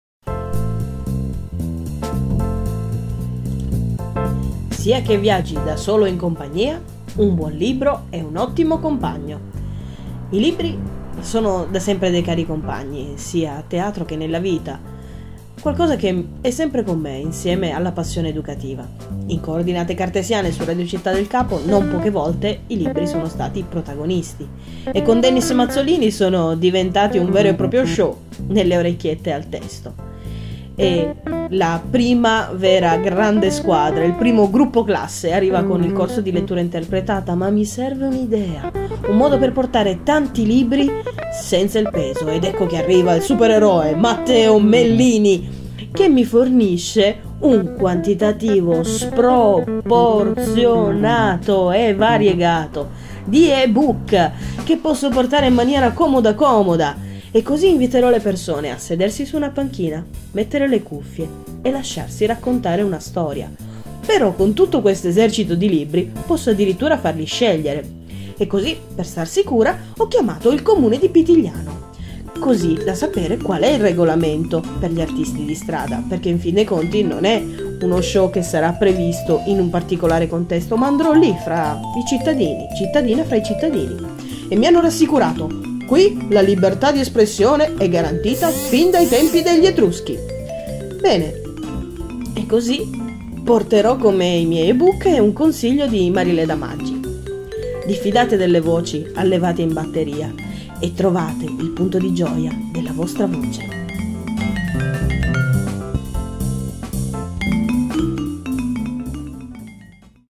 Statale 74 Cap 8° Libri in viaggio e regalo ai cittadini
I libri sono degli ottimi compagni di viaggio, e nel viaggio della vita, oltre ad averli amati, consumati, perduti e ritrovati, adesso in questo viaggio voglio farne un dono a coloro che avranno voglia di sedersi su una panchina e farsi raccontare una storia.
Ospiti speciali involontari Matteo Mellini Den Mazzolini
Citata sul finale Marileda Maggi